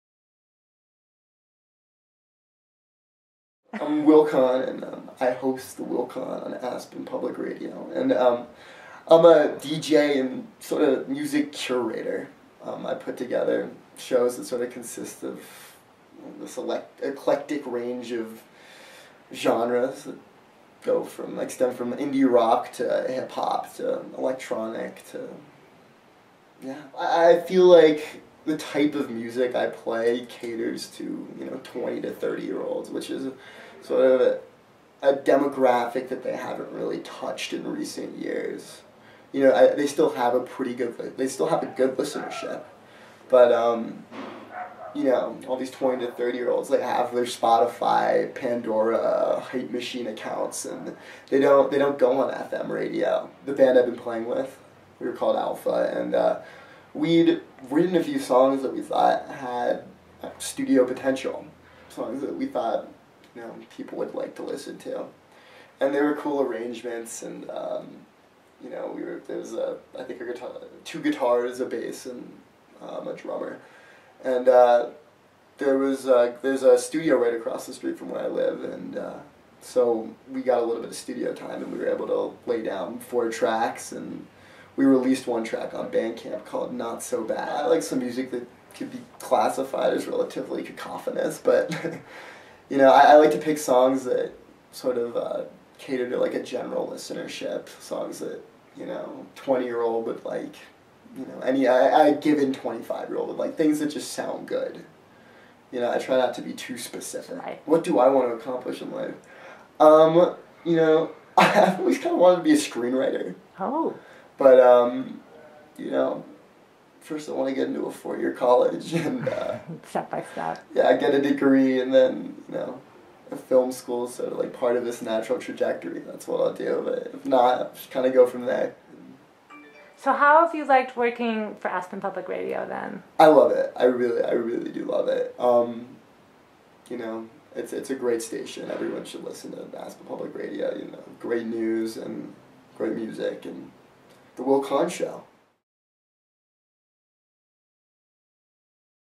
The Will Kahn Show
An interview with Will Kahn, of the Will Kahn Show on Aspen Public Radio, shows the diversity and talent of teens who grow up in a cool mountain community.